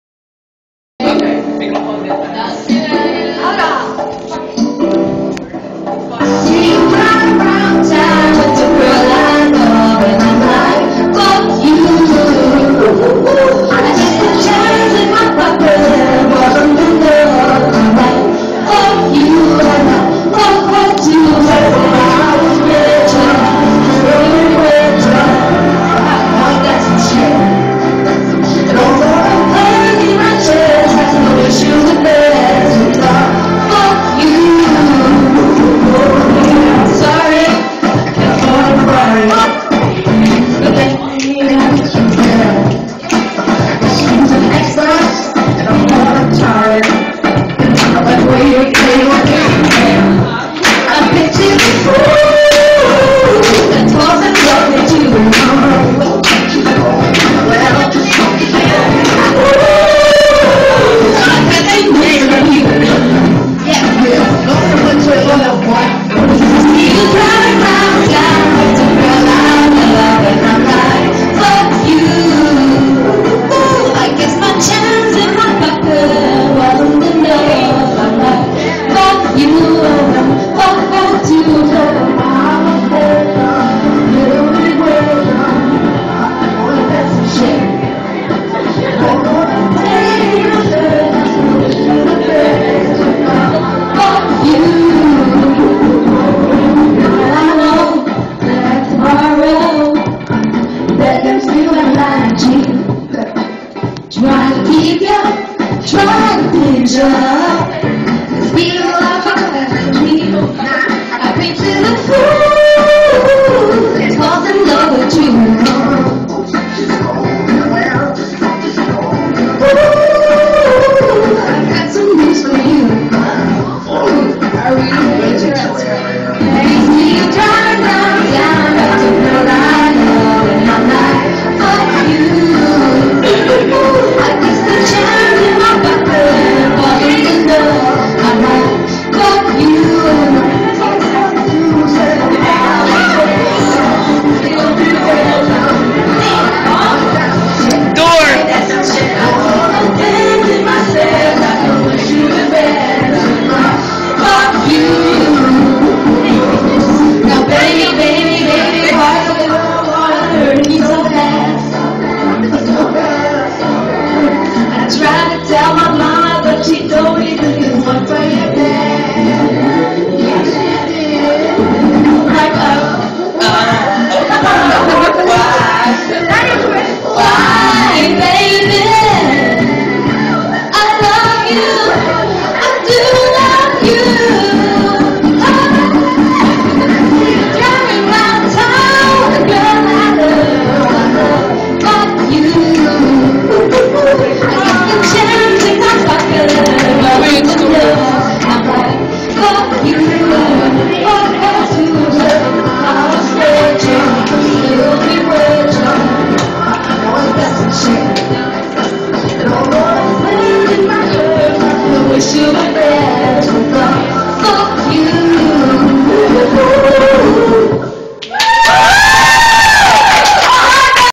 Laura Sterkenburg
Heres Laura Sterkenburg singing in the Music-lesson. This is very good. And yes I know, before people complain about a bad camera, its filmed by a cell phone in a noisy room with children in it.
Laura Sterkenburg is 12 years old and Dutch. Ignore the sound on the background, please. Thumbs up!!